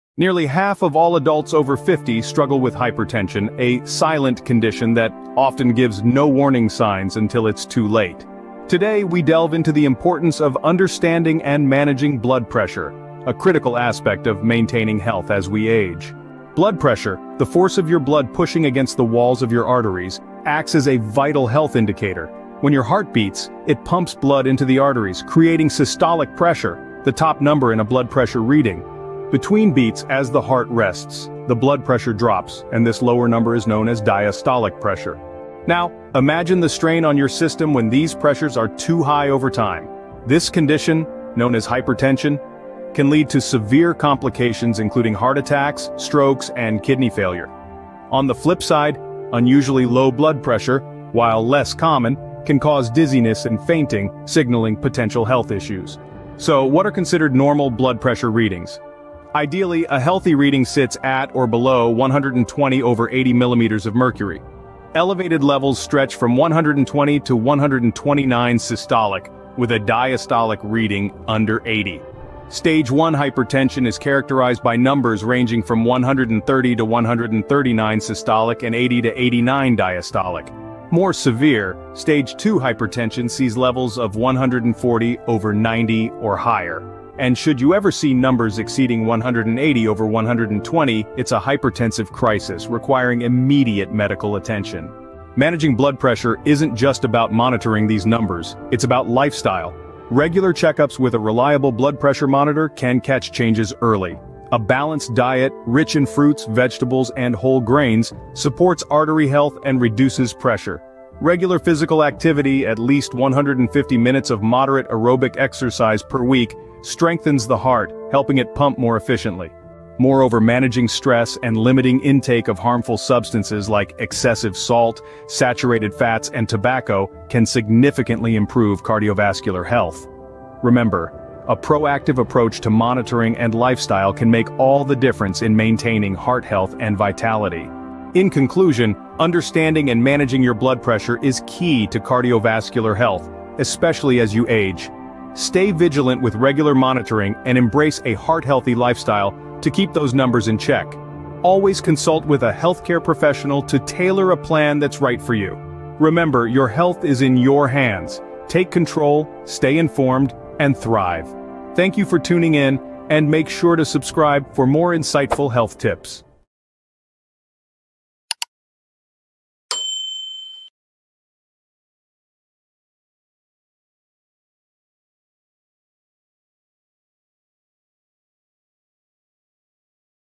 How Can You Read Blood Pressure Properly
Unlock the secrets to understanding blood pressure readings and effective management strategies in our latest video! Learn how to accurately interpret blood pressure measurements and discover practical tips for maintaining optimal cardiovascular health. Whether you're new to monitoring blood pressure or looking to enhance your management approach, this video has valuable insights for everyone. Don't miss out—watch now and take control of your heart health!

OUTLINE:

00:00:00 The Importance of Blood Pressure Awareness
00:00:16 Understanding Blood Pressure
00:00:35 Impact of High and Low Blood Pressure
00:00:56 Normal and Abnormal Blood Pressure Readings
00:01:35 Managing Blood Pressure Through Lifestyle
00:01:59 Additional Tips for Cardiovascular Health
00:02:17 Conclusion and Call to Action